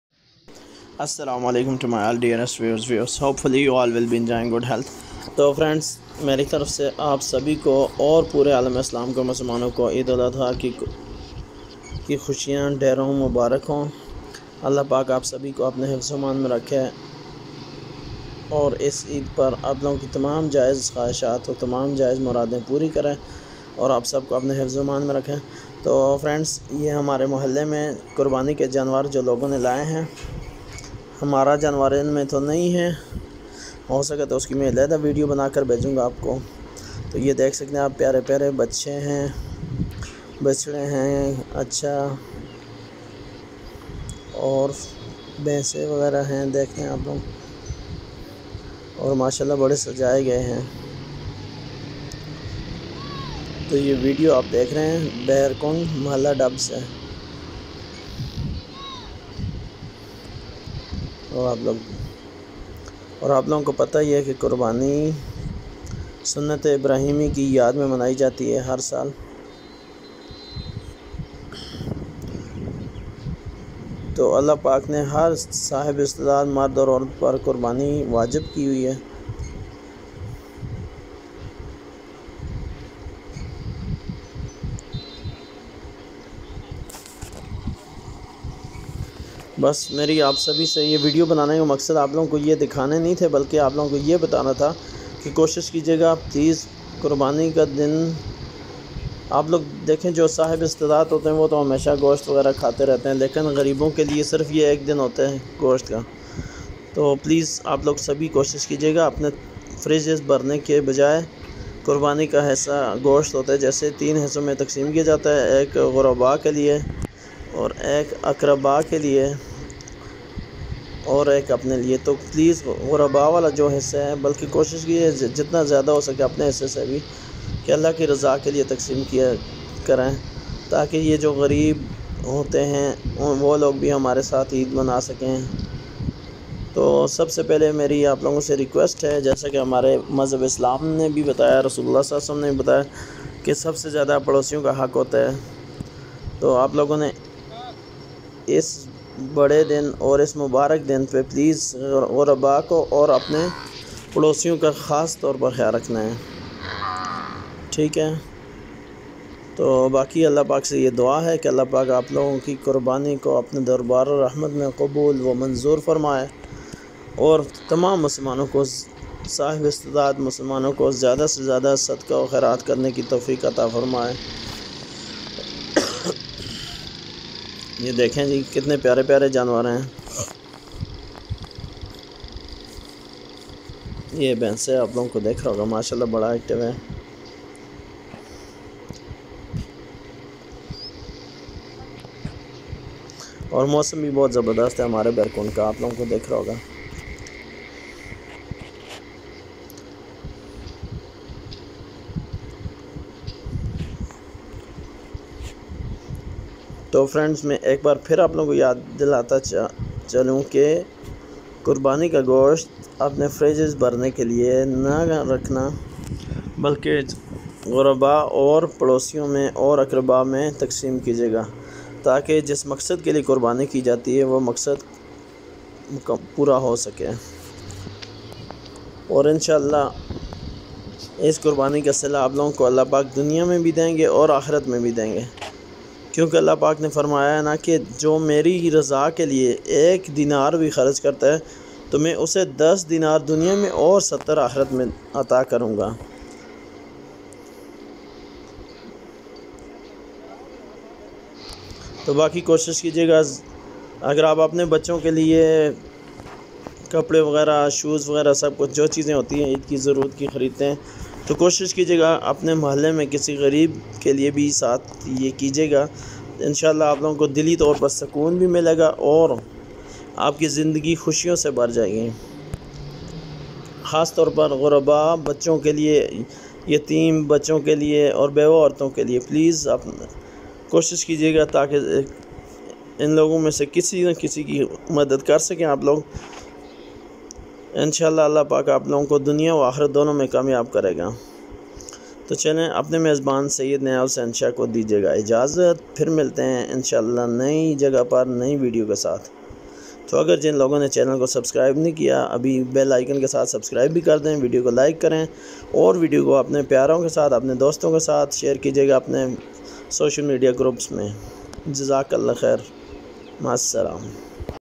Qurbani 2021 || Humare Mohallay Qurbani K Liye Laye Gaye Janwar || Village Life & Tours
In This video I will show you the Beautiful Animals in our Village brought for slaughtering in repeating the Sunah of Prophet Ibrahim also Known as I raham